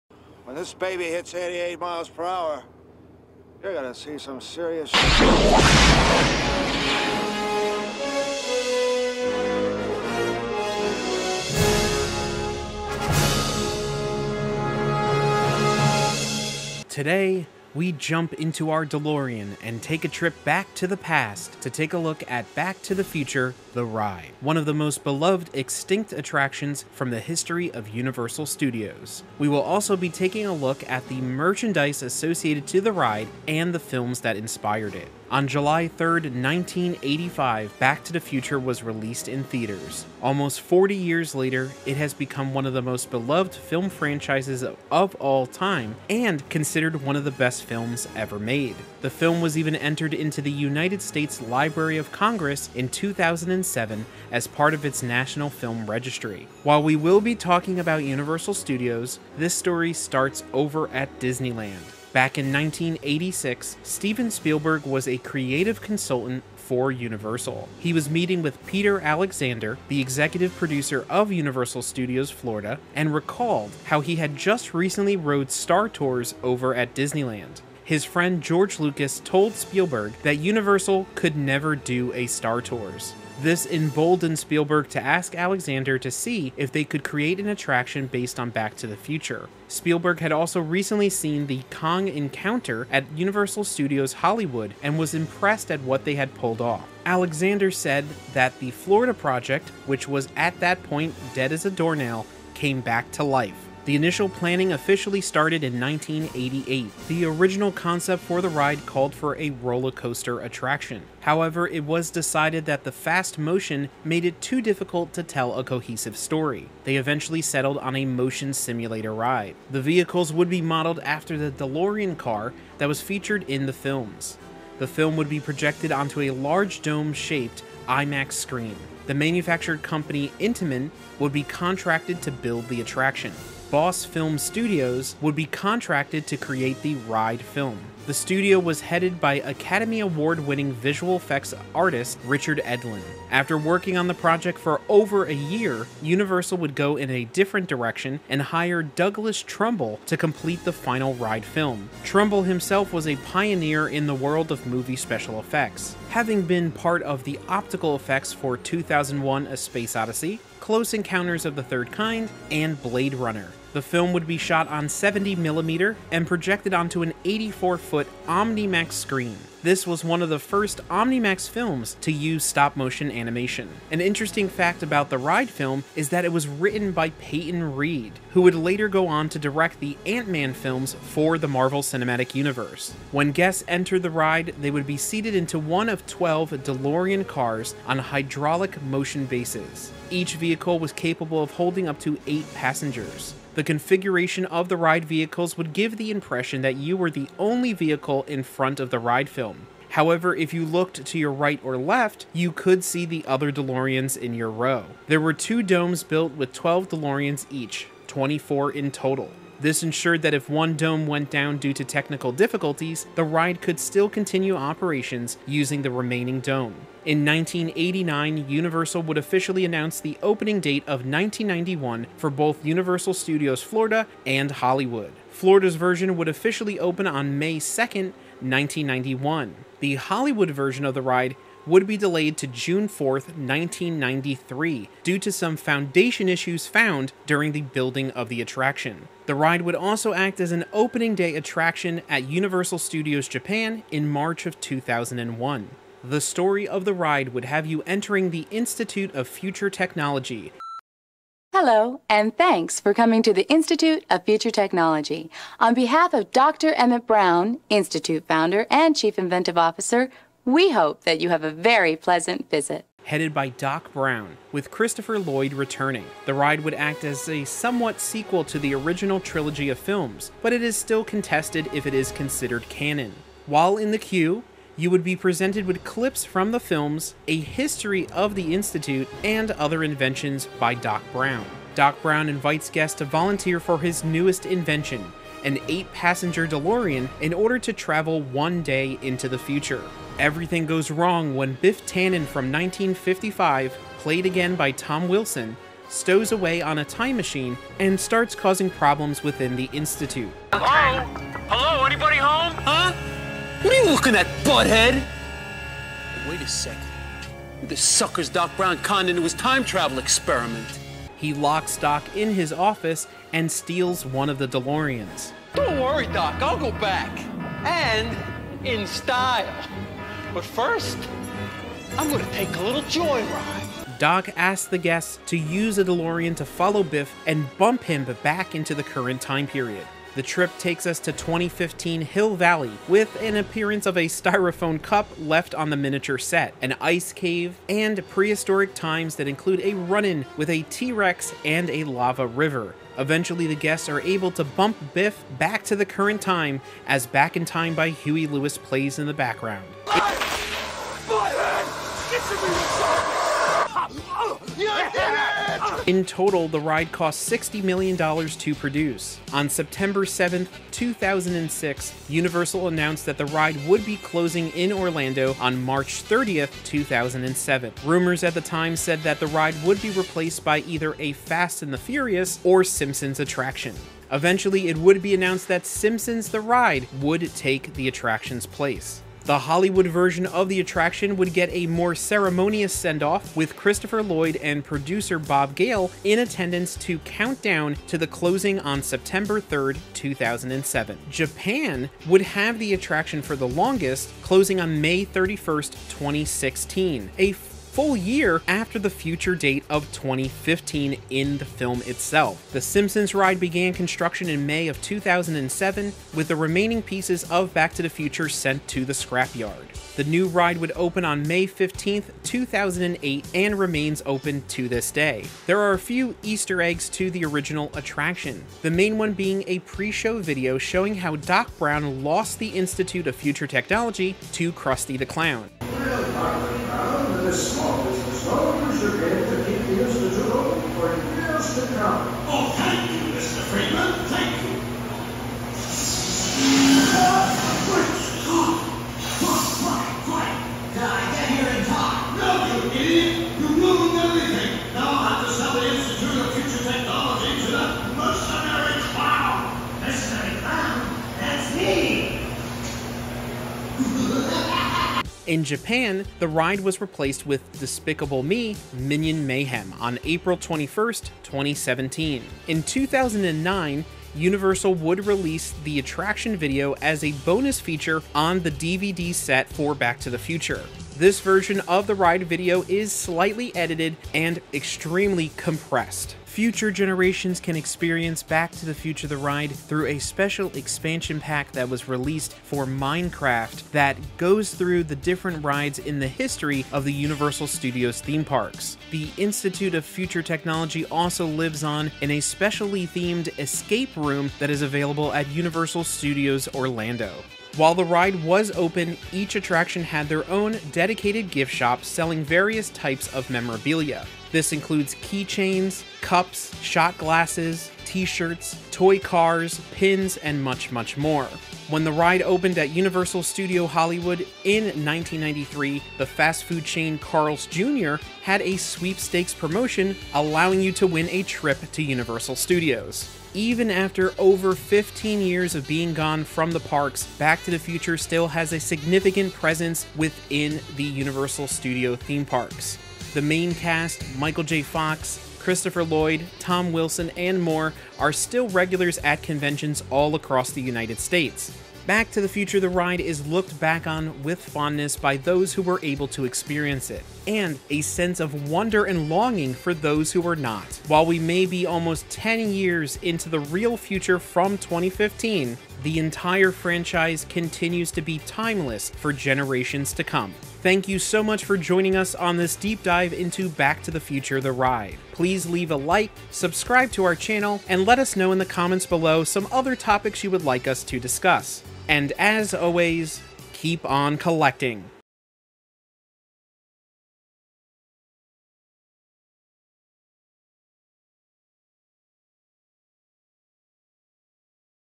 Back to the Future: The Ride - A History
The Hidden History of Back to the Future: The Ride!

Take a thrilling journey through time as we explore the history of the iconic Back to the Future Ride! From its groundbreaking debut to its unforgettable closure, discover the magic behind this classic attraction. Buckle up, because this is one ride you won't want to miss!

🕘 Timestamps
00:00 Intro
00:35 The Film
00:56 Ride Backstory
01:43 Making of the Ride
02:13 Ride Film Creation
03:03 Ride Vehicle
03:37 Ride Announcement
04:06 Ride Experience
05:59 Ride Closure
06:42 Simpsons Ride
07:42 Ride Legacy